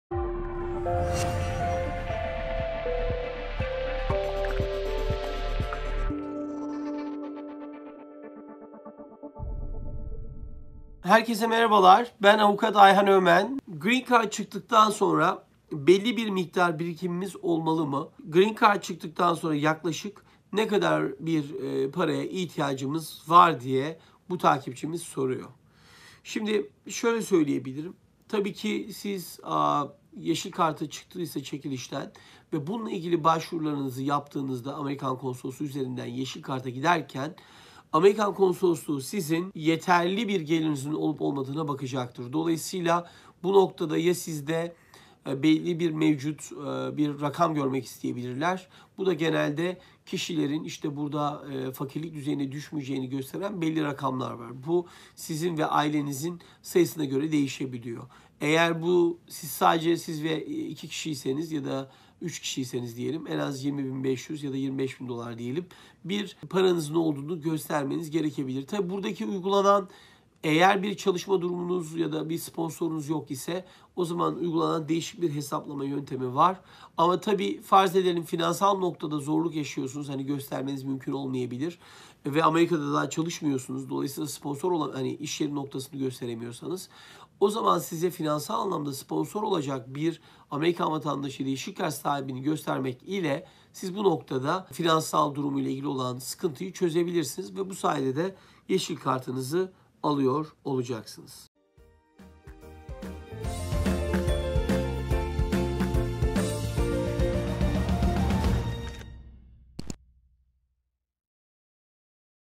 GREEN CARD ÇIKTIKTAN SONRA NE KADAR PARAYA SAHİP OLUNMALIDIR ?
Soru: Green Card çıktıktan sonra belli bir miktar birikimimiz olmalı mı ? Green Card çıktıktan sonra yaklaşık kaç paraya ihtiyacımız var ?